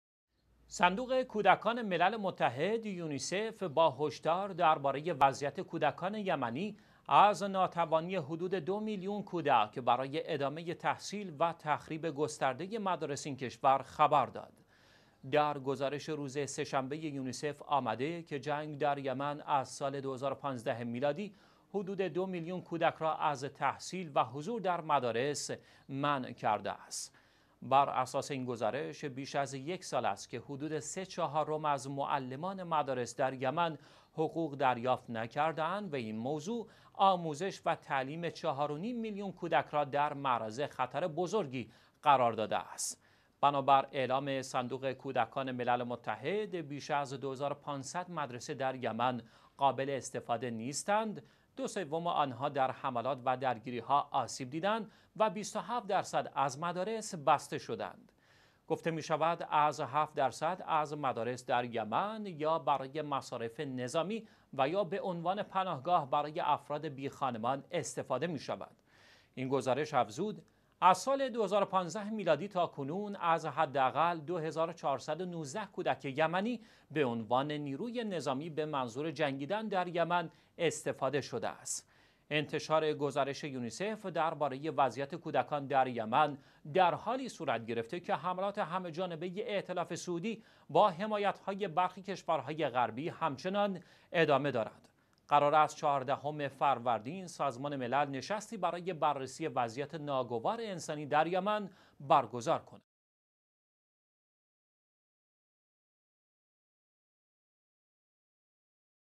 گزارش تازه یونیسف درباره وضعیت ناگوار کودکان یمنی
صندوق کودکان سازمان_ملل متحد (یونیسف) با هشدار درباره وضعیت کودکان یمنی، از ناتوانی حدود دو میلیون کودک برای ادامه تحصیل و تخریب گسترده مدارس این کشور خبر داد.

در گزارش امروز سه شنبه یونیسف آمده که جنگ در یمن از سال 2015، حدود 2 میلیون کودک را از تحصیل و حضور در مدارس منع کرده است.

بر اساس این گزارش، بیش از یک سال است که حدود سه چهارم از معلمان مدارس در یمن حقوق دریافت نکرده‌اند و این موضوع، آموزش و تعلیم 4.5 میلیون کودک را در معرض خطر بزرگی قرار داده است.

بنا بر اعلام صندوق کودکان ملل متحد، بیش از 2500 مدرسه در یمن قابل استفاده نیستند، دو سوم آنها در حملات و درگیری‌ها آسیب دیدند و 27 درصد از مدارس بسته شدند.

گفته می‌شود که از 7 درصد از مدارس در یمن یا برای مصارف نظامی و یا به عنوان پناهگاه برای افراد بیخانمان استفاده می‌شود.

این گزارش افزود: از سال 2015 تاکنون، از حداقل 2419 کودک یمنی به عنوان نیروی نظامی به منظور جنگیدن در یمن استفاده شده است.

انتشار گزارش یونیسف درباره وضعیت کودکان در یمن در حالی صورت گرفته که حملات همه جانبه ائتلاف سعودی، با حمایت های برخی کشورهای غربی همچنان ادامه دارد.

قرار است 14 فروردین سازمان ملل نشستی برای بررسی وضعیت ناگوار انسانی در یمن برگزار کند.